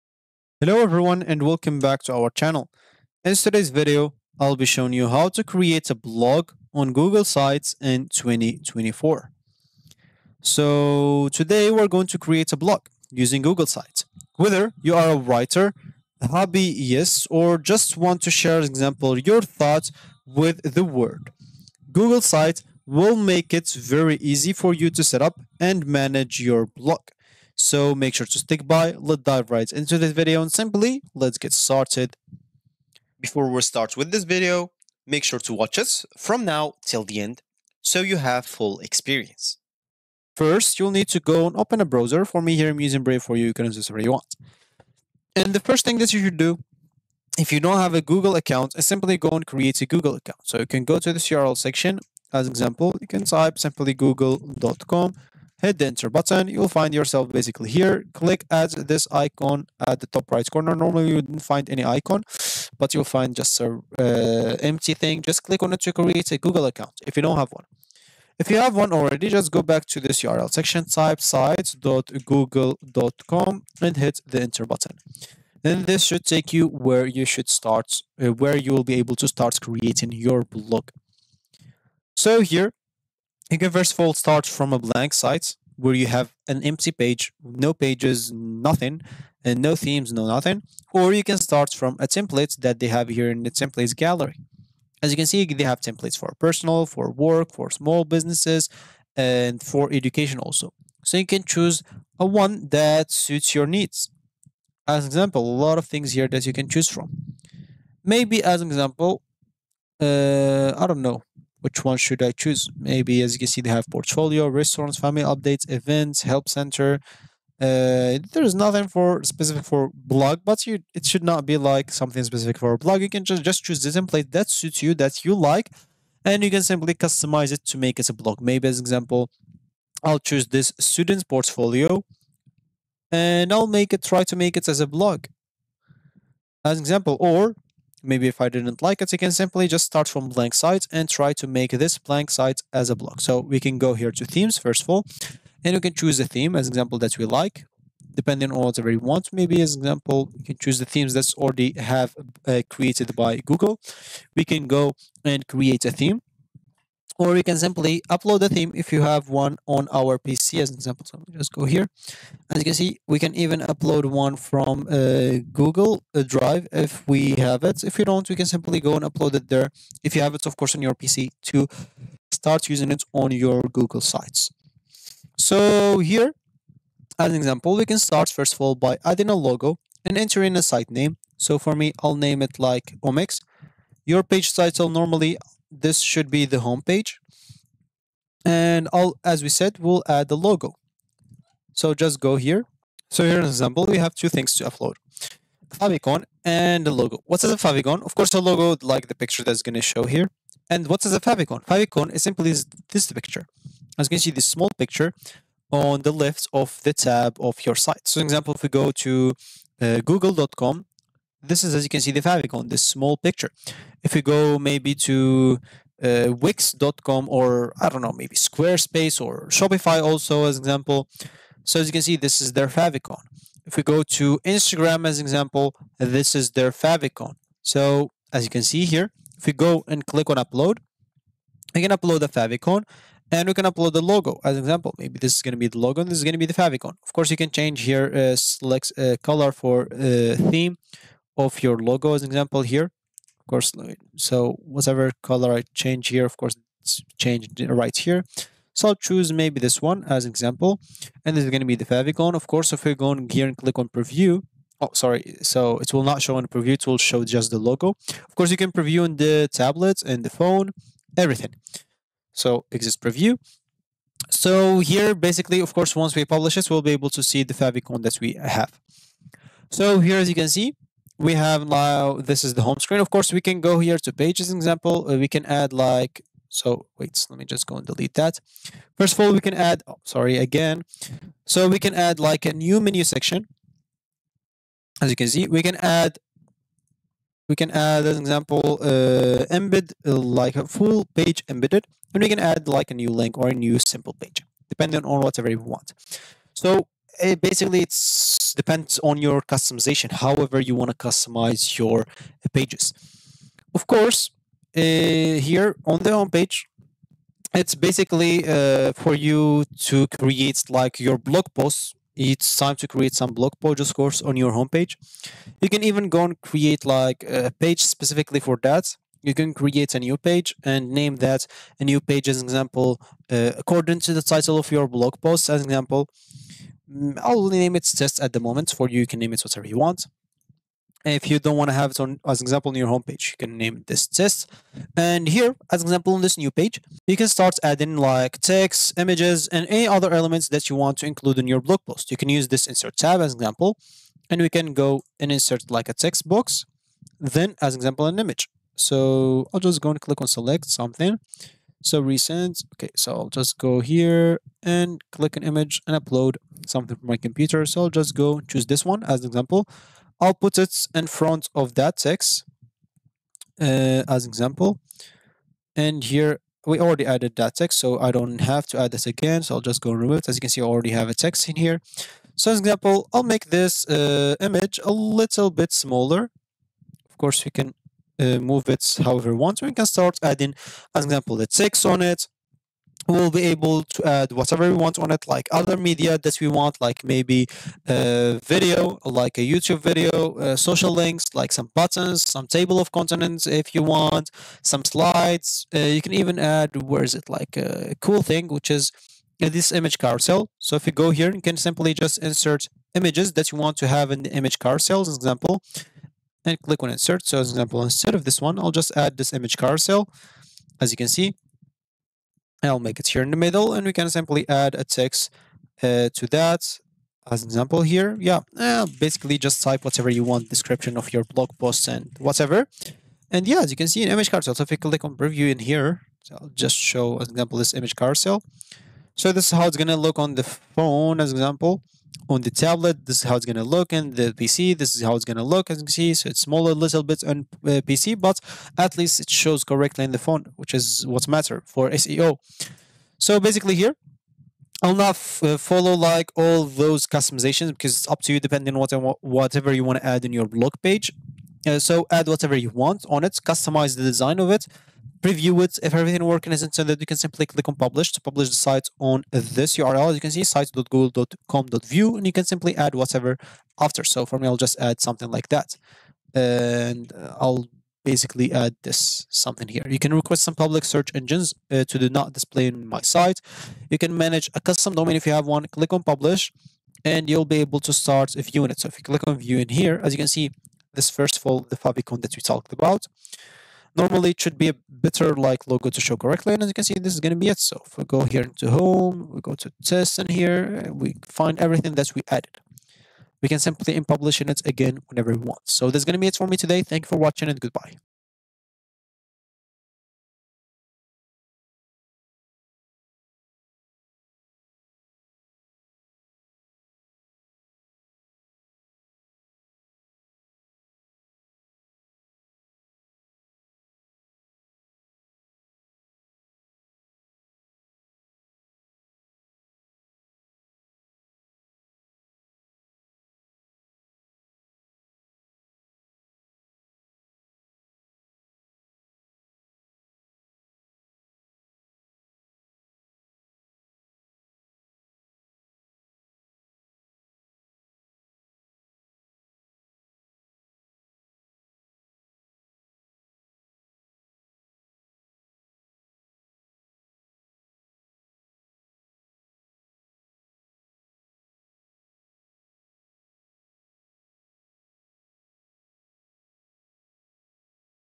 How To Create a Blog on Google Sites 2024! (Full Tutorial)
Today we talk about create a blog on google sites,google sites tutorial,google sites tips and tricks,google sites blog,google sites blog tutorial